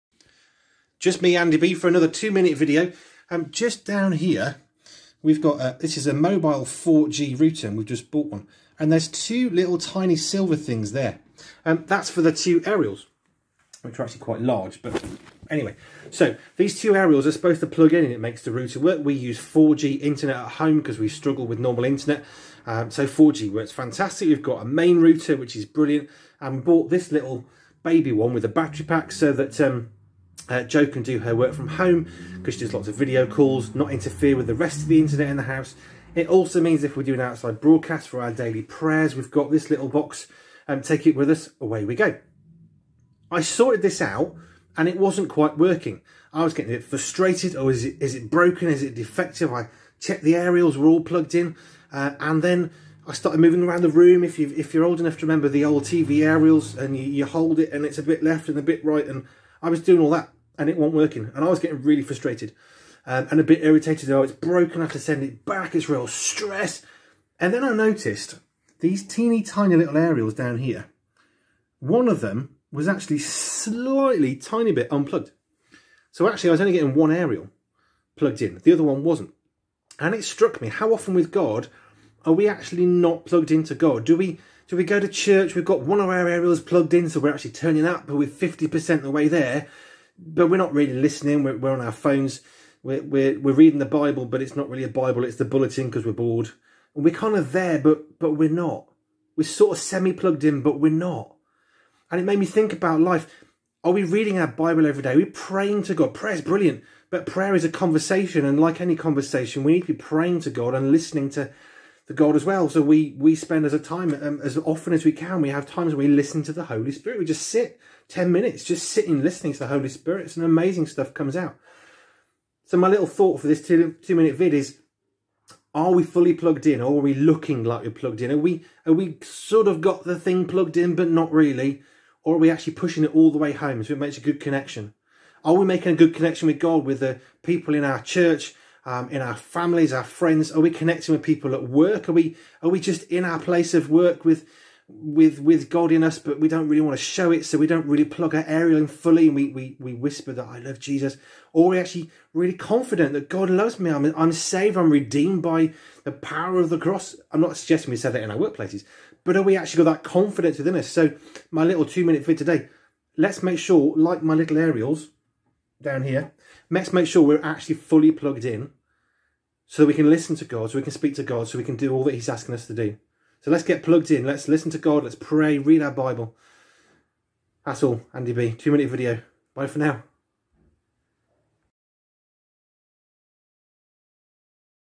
Andy B, 2 minute video, 50% Connected Is Not Fully Connected, S1, E1
This video can be downloaded and streamed in your church services or at home.

How connected are we to God?

Are we fully connected to God and the people around us, or just partly - do we look more connected than we really are?

Andy B